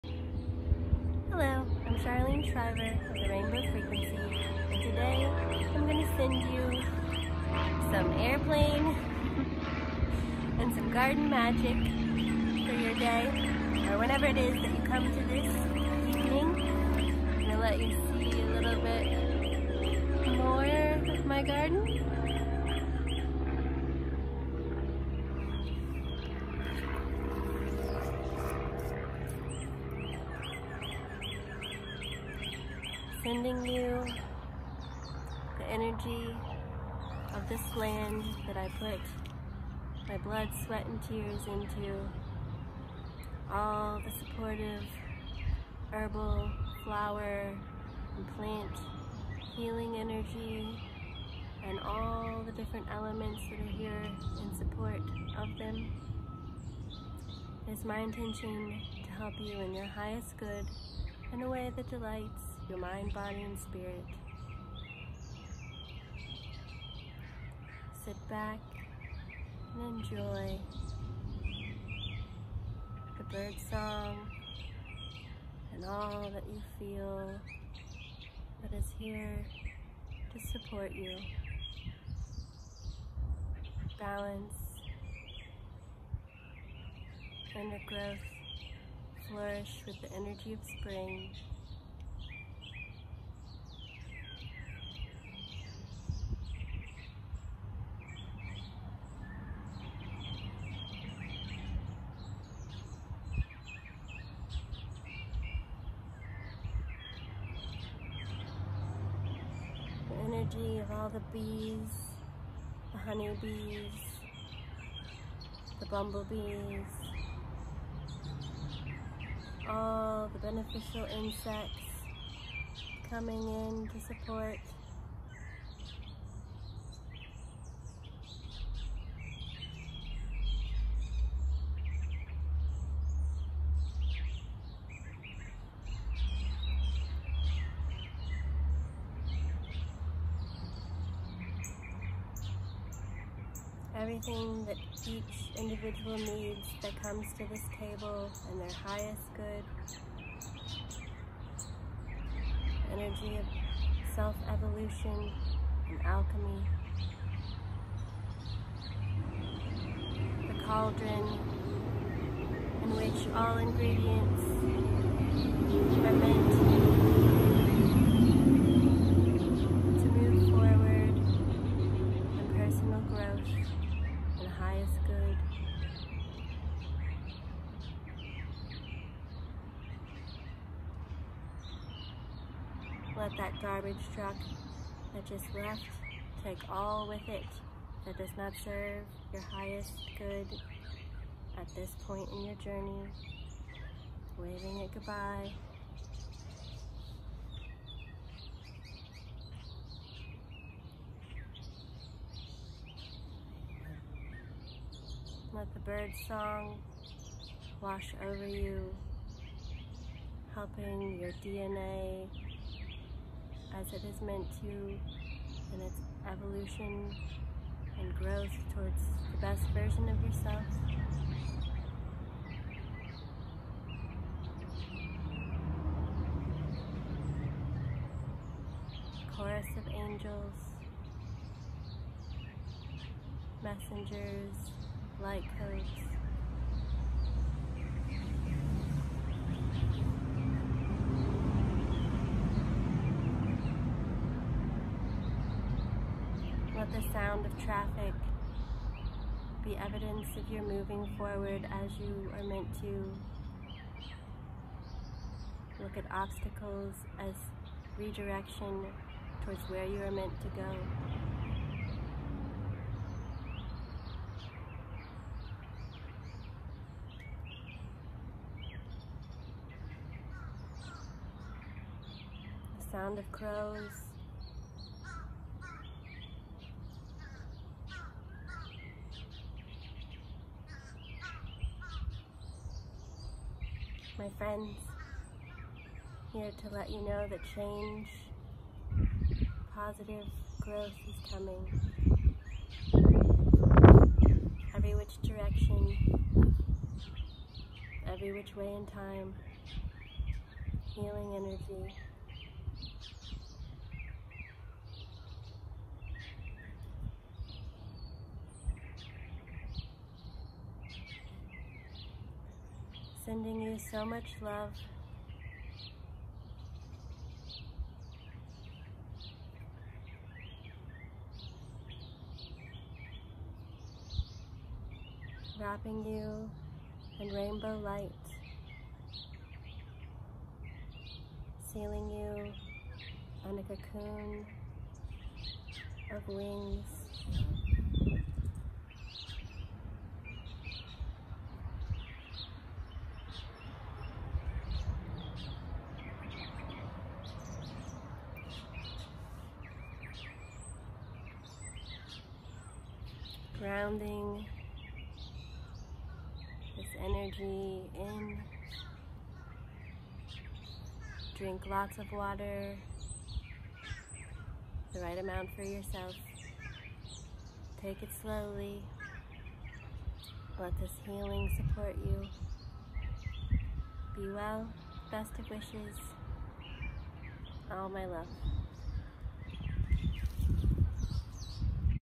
Faery Garden Energy For Holistic Wellness
Welcome back to my garden for some energy balancing, restoration, and holistic wellness that is done with the intention of aiding you in your highest good in a way that delights your mind, body, and spirit!

I am a conduit for energy reading and balancing as well as an intuitive empath and spiritual medium.

use is not allowed without permission.